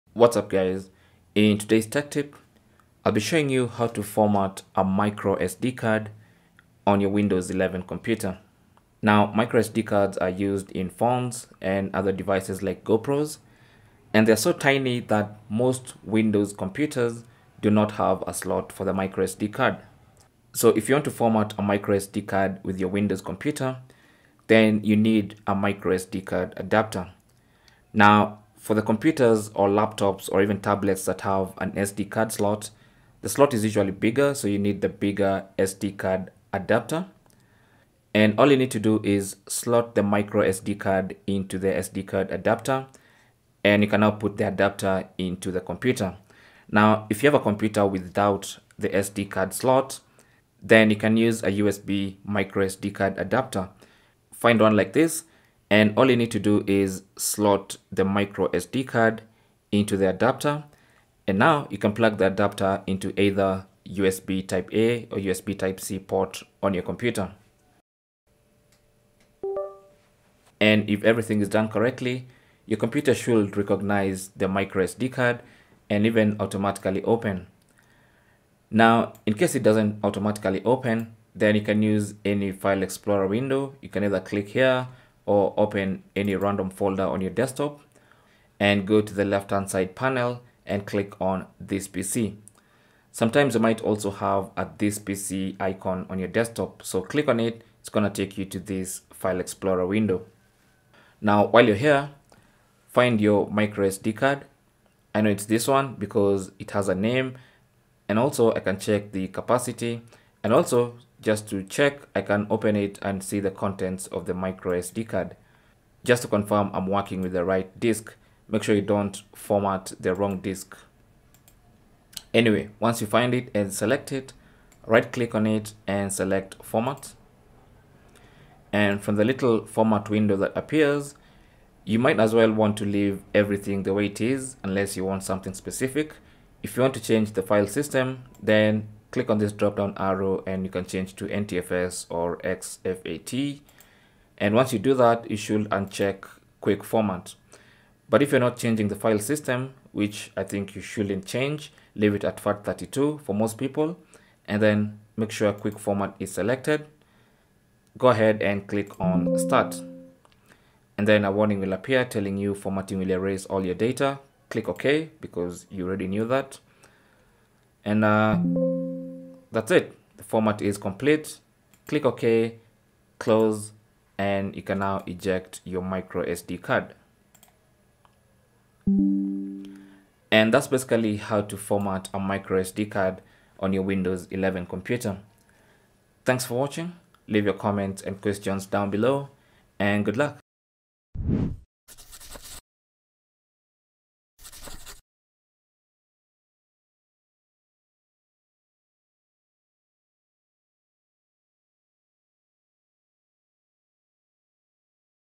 How To Format Micro SD Card On Windows 11
Here's how to format a micro SD card on Windows 11.
Using an SD card adapter, you can format a micro SD card by plugging it into a laptop or PC. Once you format, all data within the card will be permanently erased.

How To Format Micro SD Card In Windows 11:
Insert the Micro SD card into a MicroSD to SD card adapter
Insert the SD card adapter into your PC or laptop
Back up any Important Files from the card
Right-click on the Micro SD Card and select 'Format'.
From the Format pop up Window, select 'Quick Format'
Confirm you want to Format the Micro SD card
Once complete, eject Micro SD Card

Windows 11 Specifications
Edition: Windows 11 Pro
Version: 21H2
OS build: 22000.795

Cell Phone Tripod Adapter